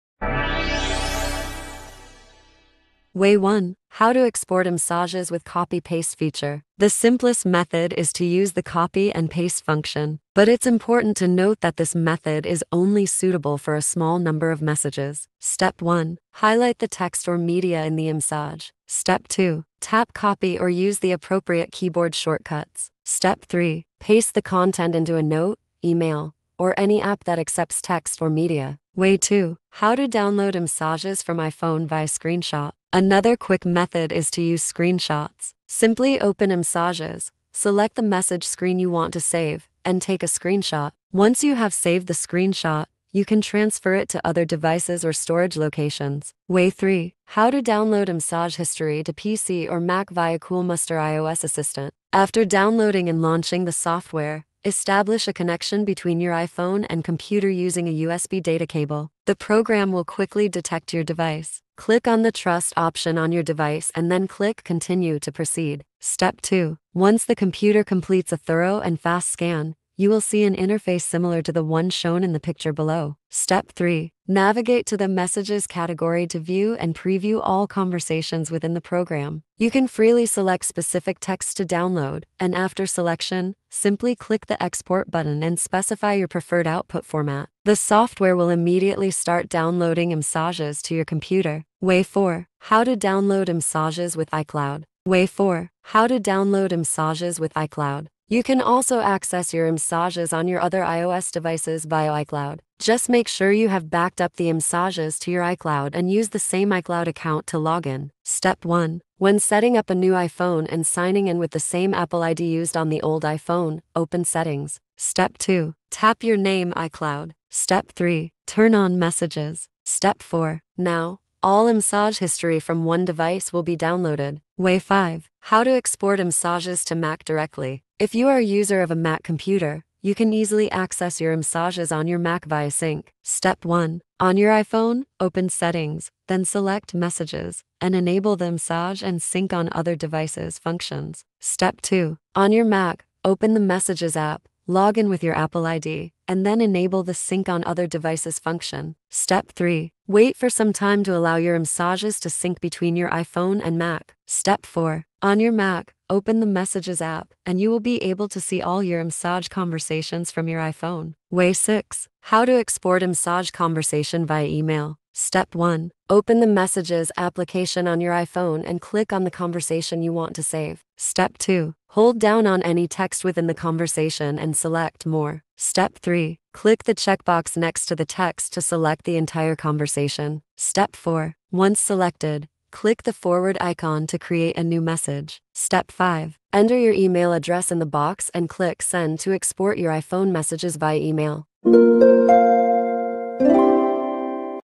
How to Download iMessages with Ease in 6 Easy and Quick Ways
Do you want to export iMessages to have a backup or for other reasons? This video introduces 6 ways on how to download iMessage.

00:00 Start
00:03 Way 1. How to Export iMessages with Copy-Paste Feature
00:28 Way 2. How to Download iMessages from iPhone via Screenshot
00:46 Way 3. How to Download iMessage History to PC/Mac via Coolmuster iOS Assistant
01:31 Way 4. How to Download iMessages with iCloud
02:04 Way 5. How to Export iMessages to Mac Directly
02:41 Way 6. How to Export iMessage Conversation via Email